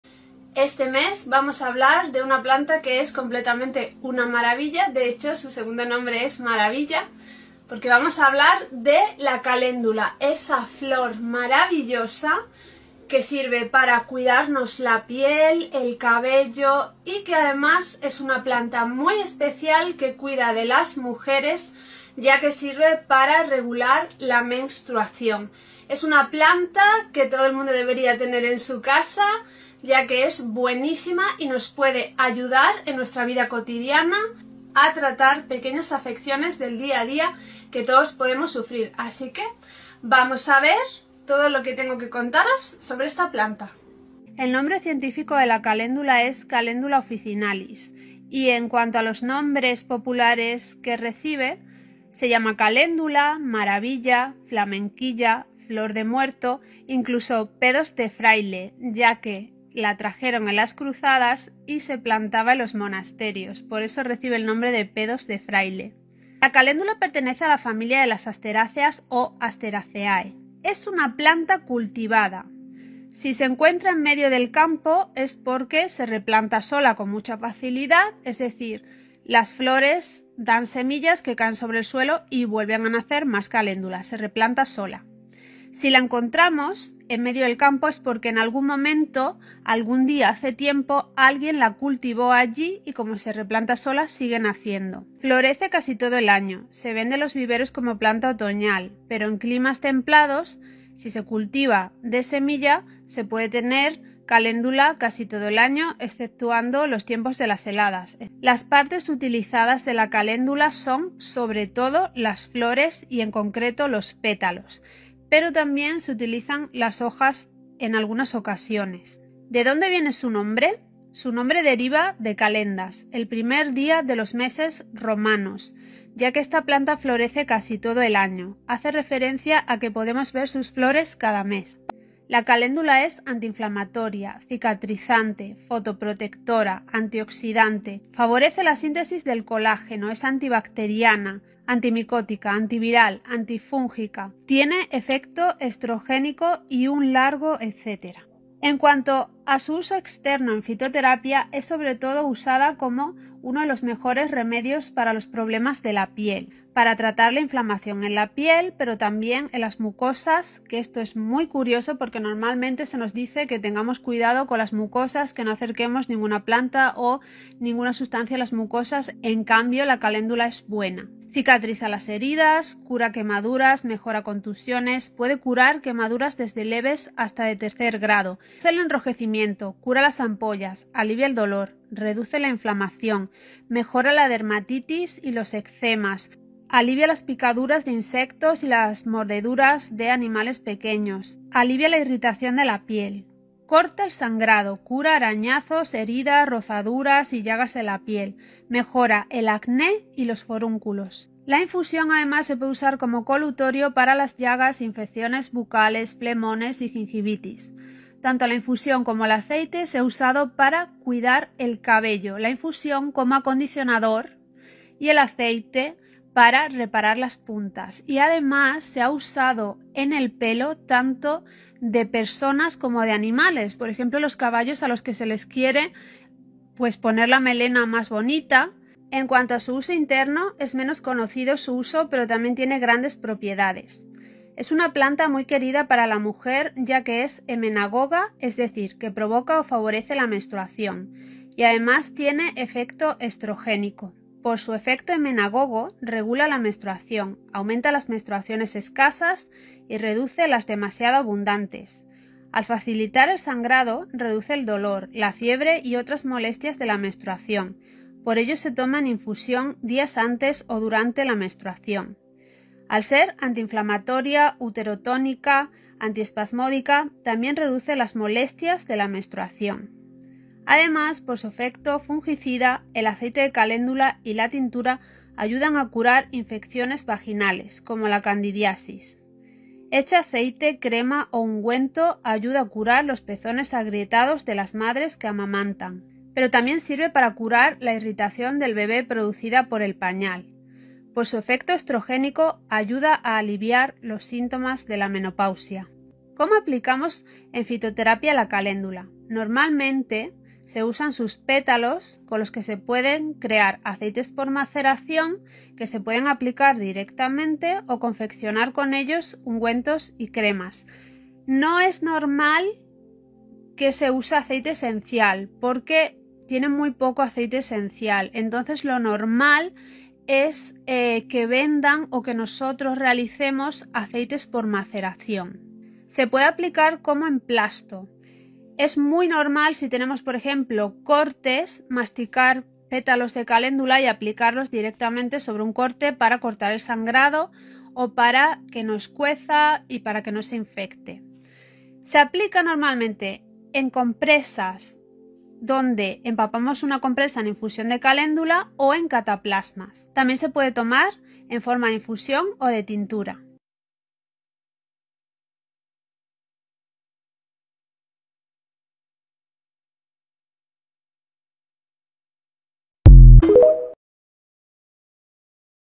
Calendula: fragmento del vídeo de Plantas de octubre en el Patreon La Bruja Verde
Os dejo un fragmento del nuevo vídeo de plantas y sus usos que subí a mi Patreon (La Bruja Verde) la semana pasada. En octubre os hablo de la caléndula, sus variedades, sus características, sobre sus usos en fitoterapia, en etnobotánica, a nivel culinario y en magia y paganismo. EL VÍDEO COMPLETO DE 18 MINUTOS ESTÁ EN MI PATREON: LA BRUJA VERDE.